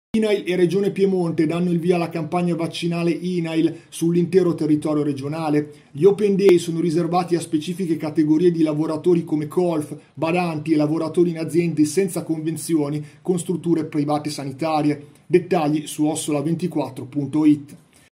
Vco 24 - Inail e Regione Piemonte: open days vaccinali per i lavoratori - (13-07-2021)
Fonte: Vco 24